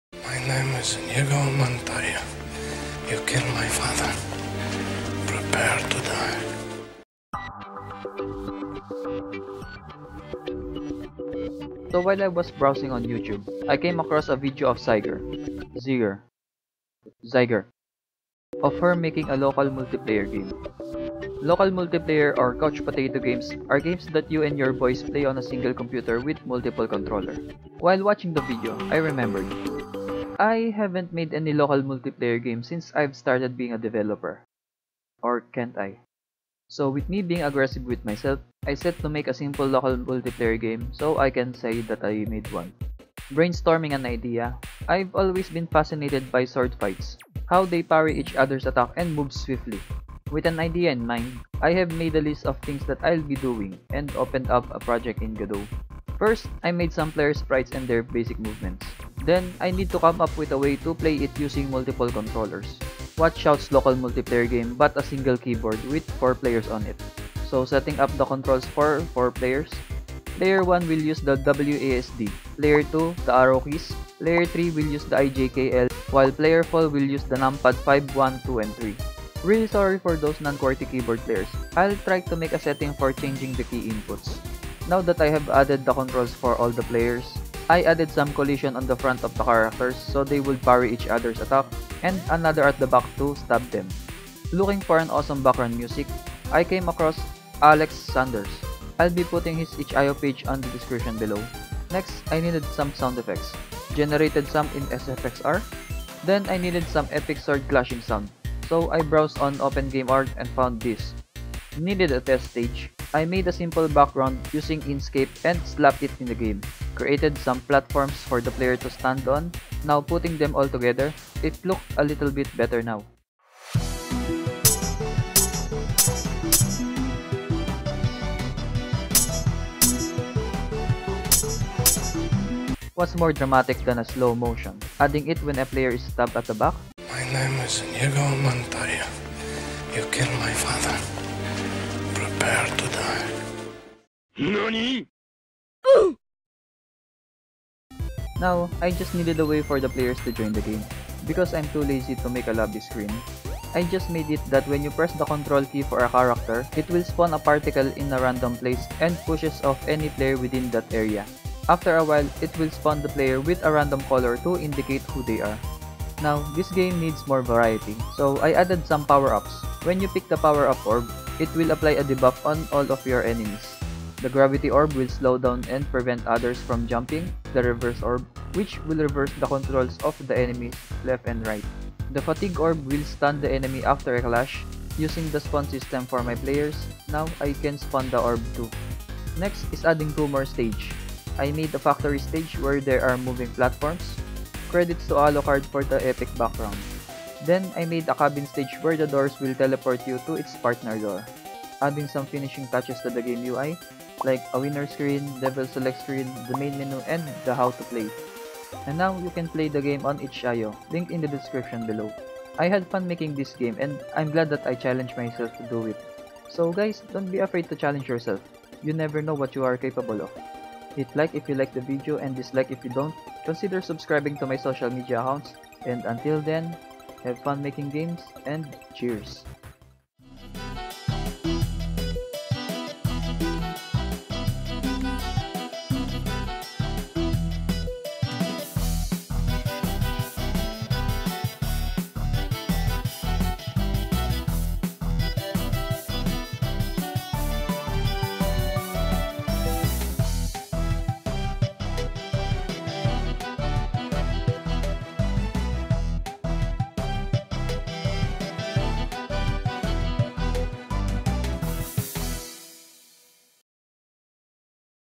Making a local co-op game because I saw one | Godot Devlog
While browsing videos on YouTube I came across a video from Zyger of her making her steam commercial local co-op game. Then I remembered that I haven't made a local co-op game before, so I challenged myself to make one.

//Credits
➤Game Bg music by Alecxs Sunders